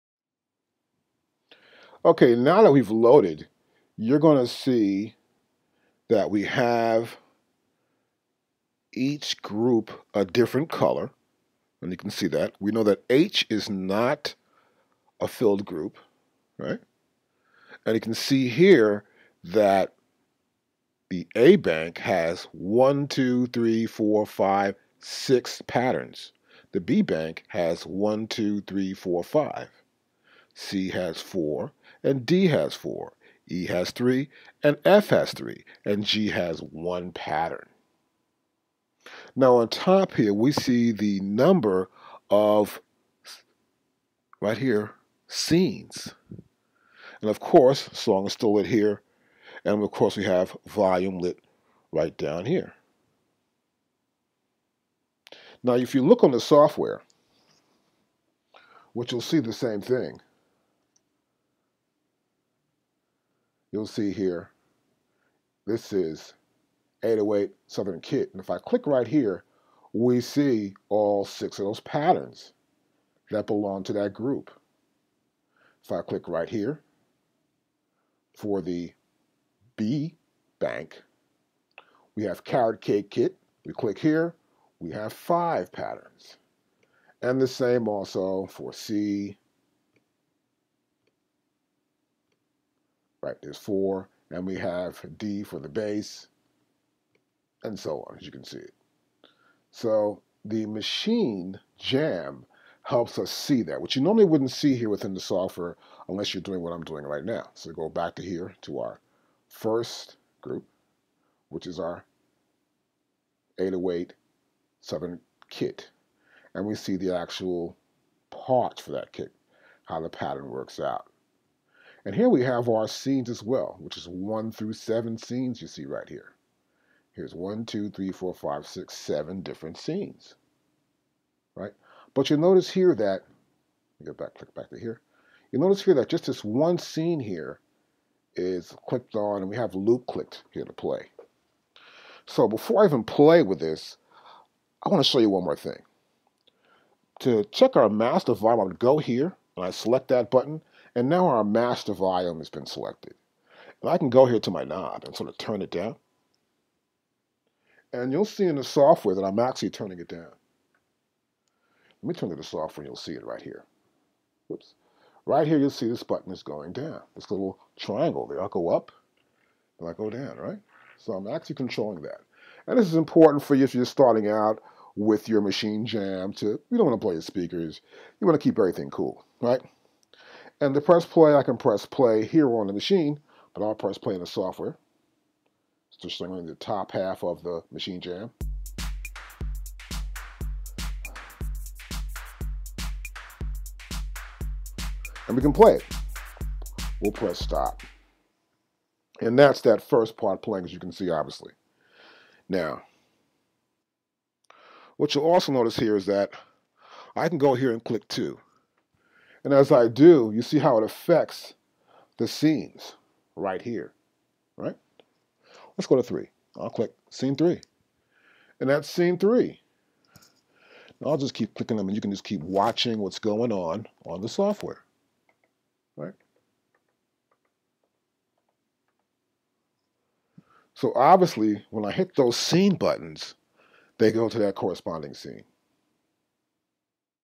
MaschineJam DVDs Lesson 2: Browse and load
MaschineJam DVDs Lesson 2: Browse and load anything directly from the Jam from our Maschine Jam Instructional DVDs @ samplekings.com